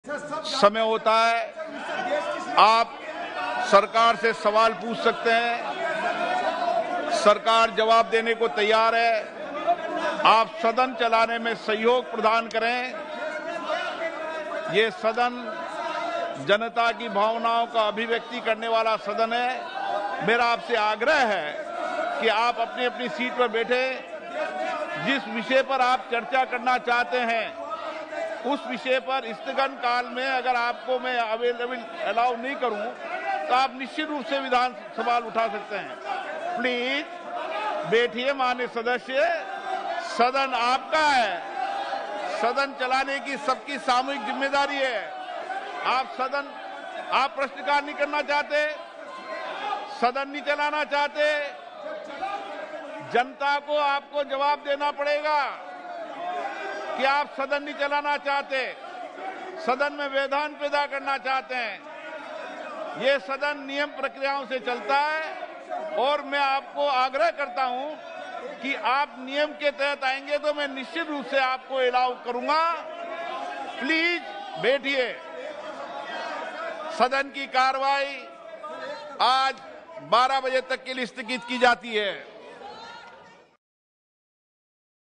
Lok Sabha Speaker Om Birla Outrage in Parliament Session | PM Modi | Sonia Gandhi | YOYO TV Kannada
LOK SABHA LIVE : PM Modi Parliament Winter Session of 17th Lok Sabha 2022 | Day 12 | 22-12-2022